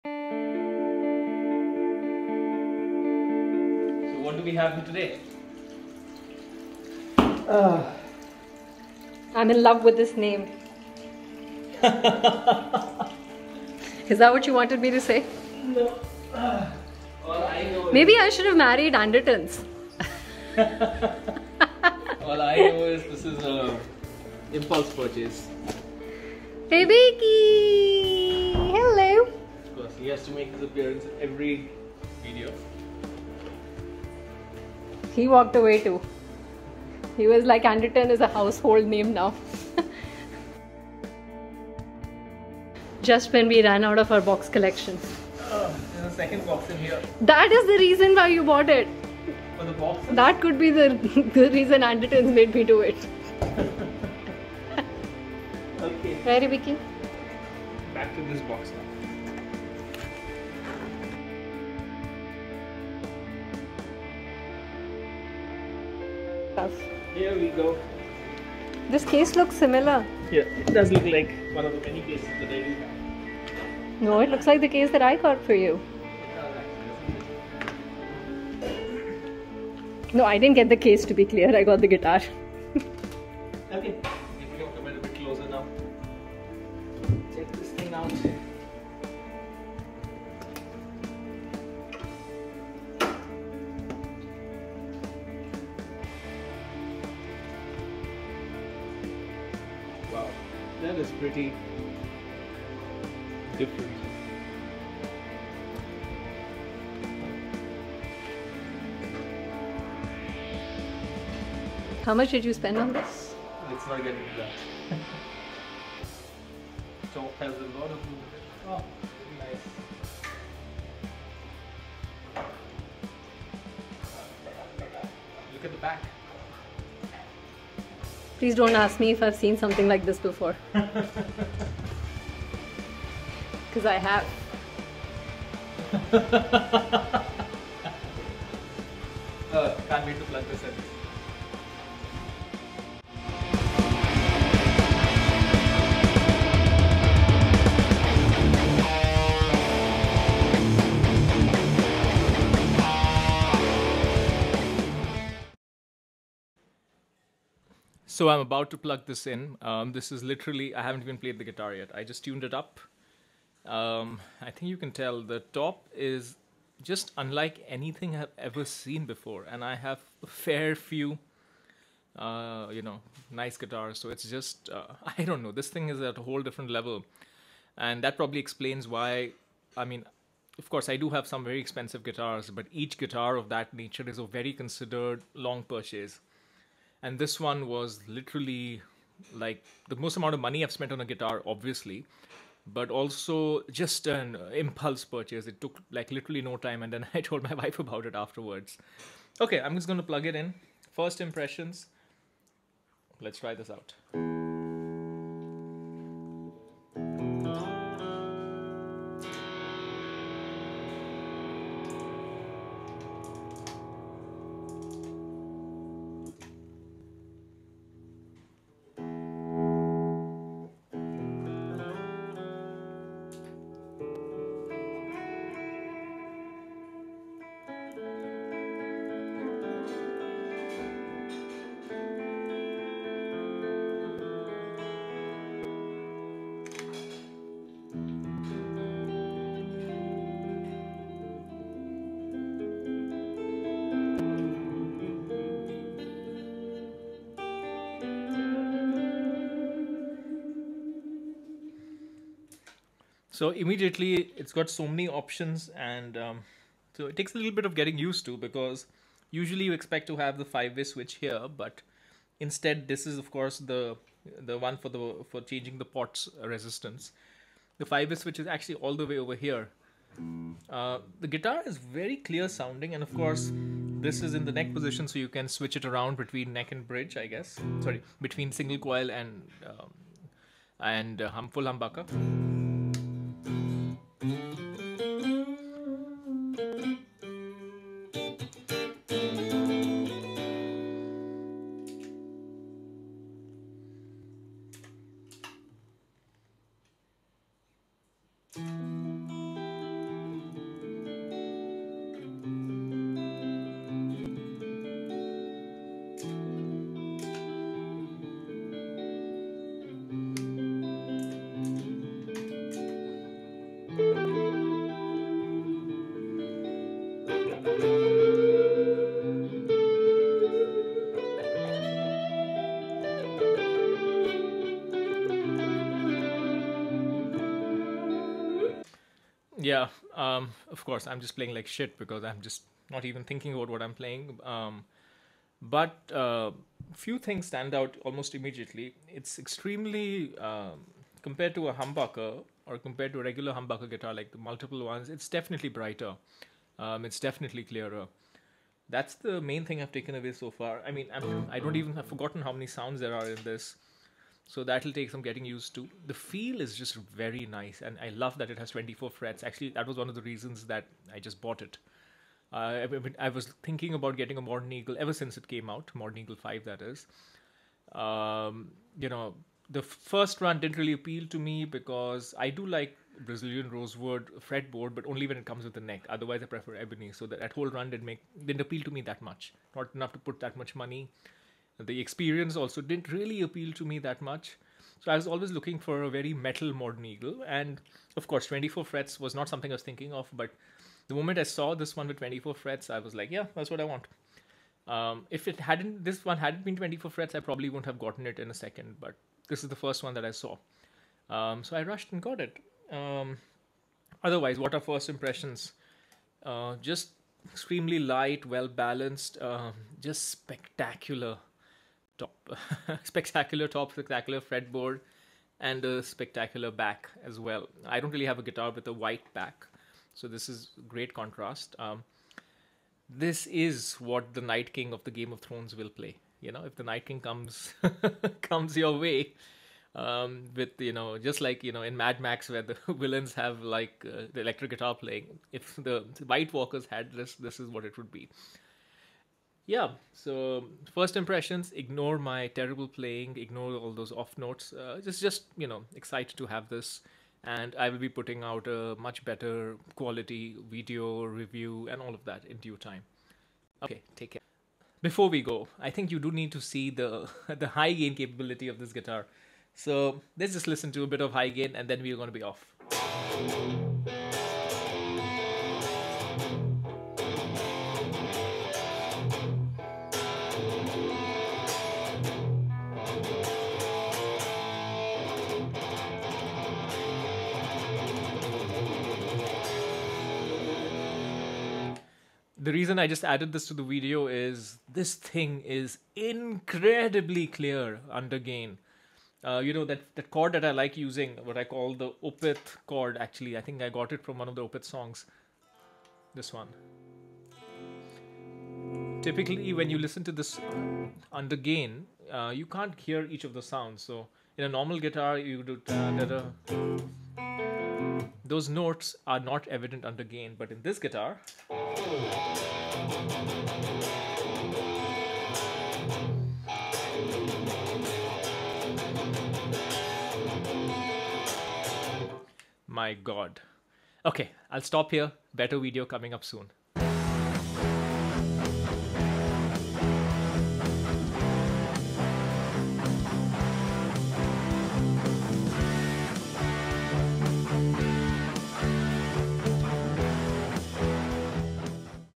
PRS Private Stock Modern Eagle V / 5 New Guitar Day: Arrival of the Night King
Unboxing and first impressions of the most expensive and impulsive purchase I have ever made in my life (but I think it was worth it). This 24 fret Modern Eagle V in Frostbite Glow and craziest quilted maple I have ever seen has me spellbound even as I write this.

Apologies that the sound levels in the video are a touch inconsistent. I was too excited to get all the technicalities sorted